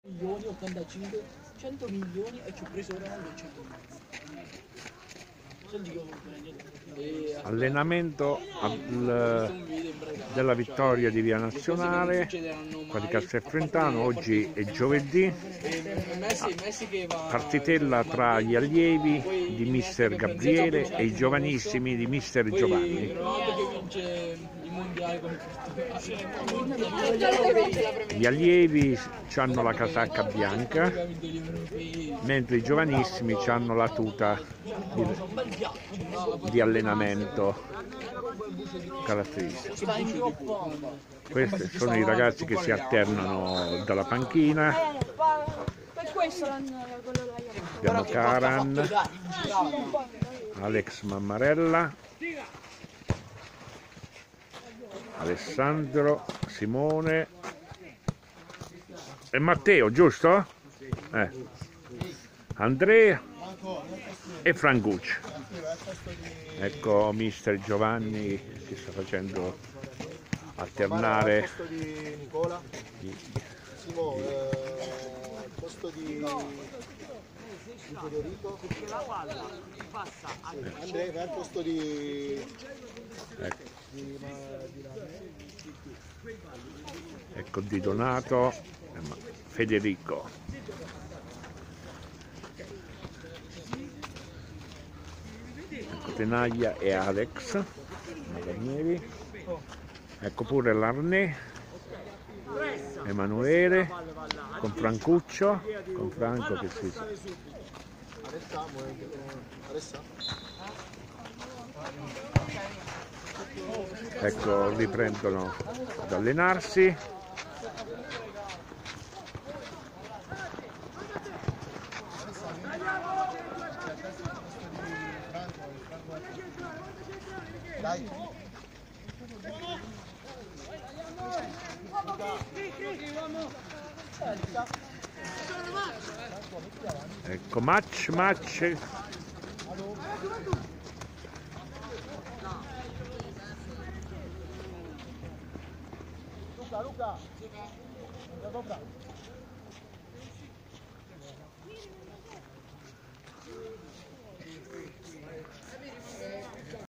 VIRTUS CASTEL FRENTANO ALLENAMENTO ALLIEVI E GIOVANISSIMI
Ieri Giovedì 11 Gennaio 2018 , ultima seduta di allenamenti della settimana  per i nostri Giovanissimi e Allievi con i rispettivi Mister Giovanni e Gabriele al " Della Vittoria " di Via Nazionale .
I ragazzi si sono ben preparati alle Gare di Campionato ,Domani  scenderanno  in Campo i Giovanissimi contro il Sambuceto Calcio , mentre Domenica mattina è il turno dei nostro Allievi contro l'Ortona Calcio .